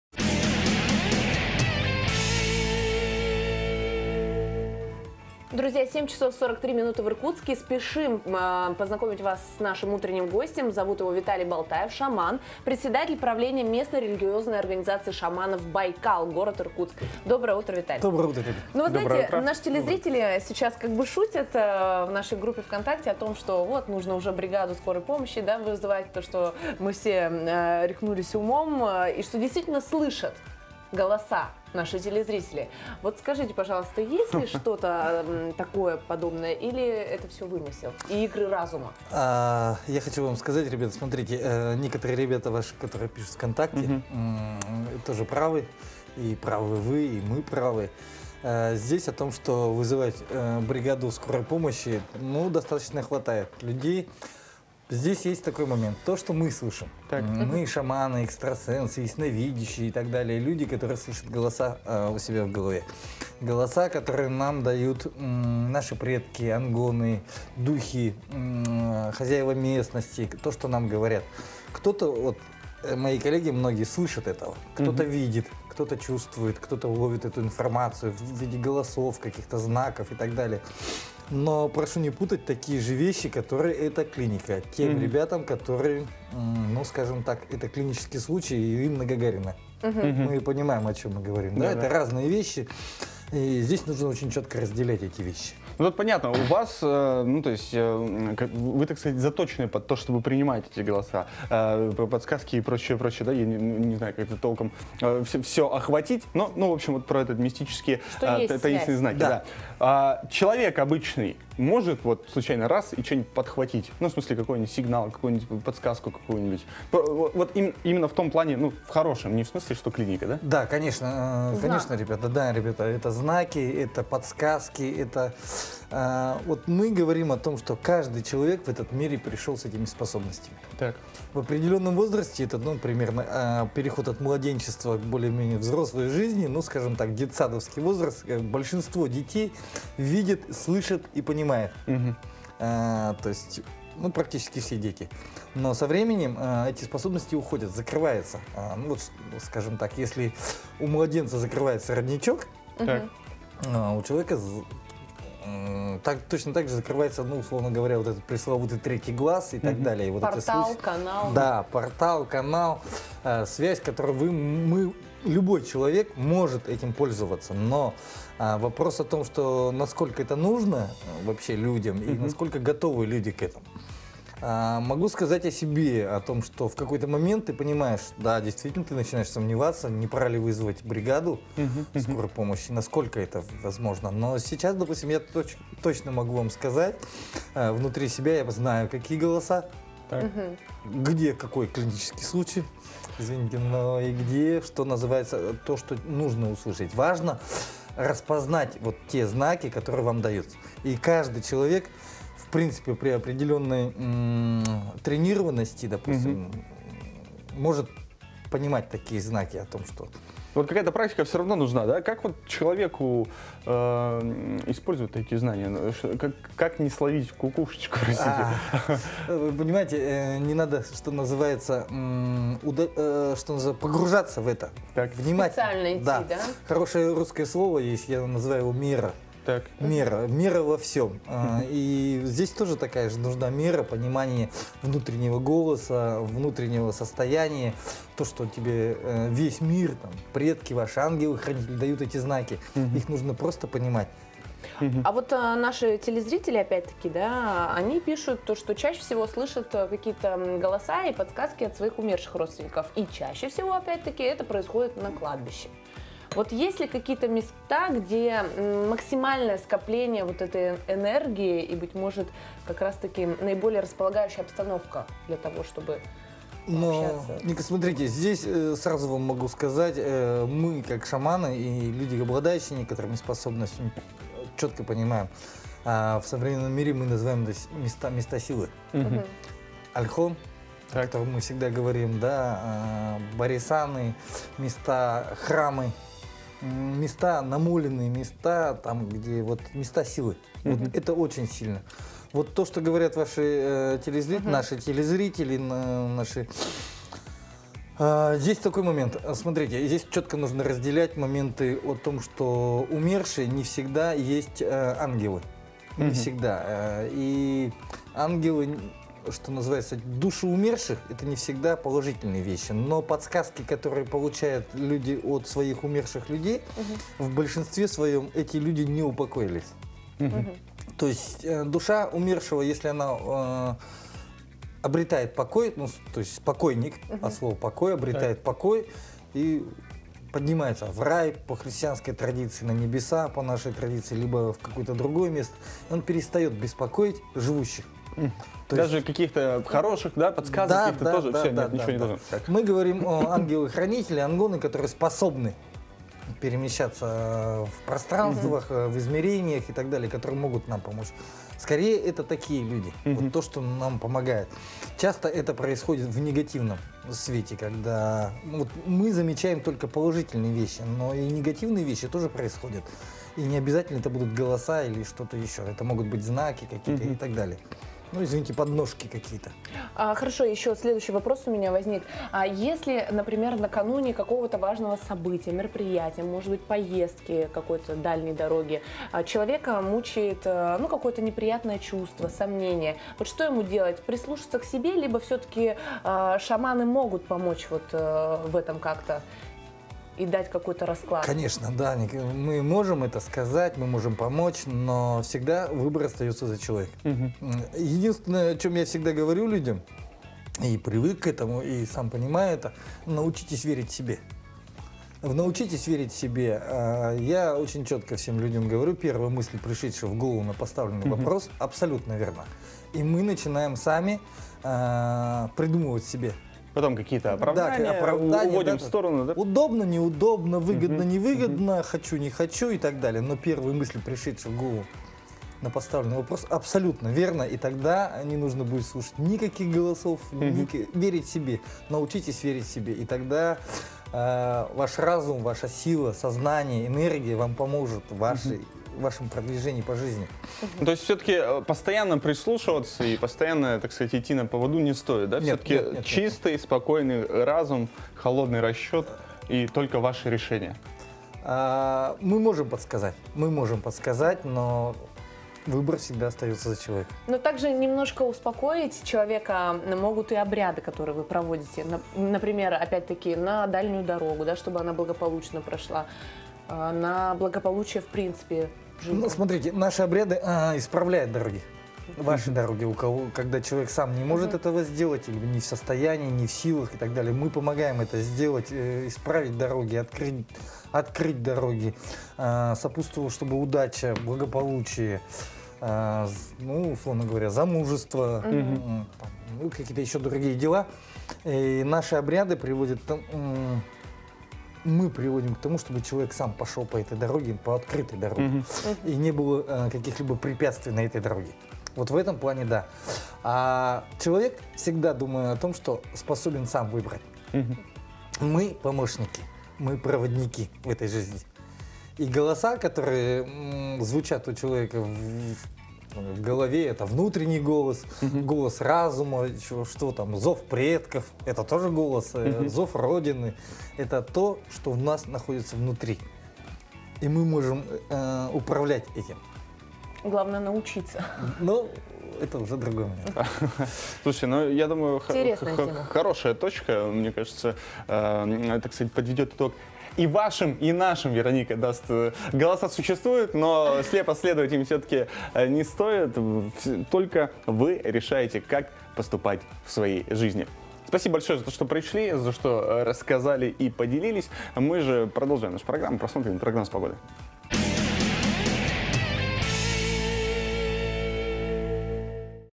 Виталий Балтаев о подсказках и знаках судьбы
Гость программы Виталий Балтаев, шаман, председатель правления МРОШ «Байкал», г. Иркутск рассказал о способностях слышать подсказки судьбы и чувствовать их.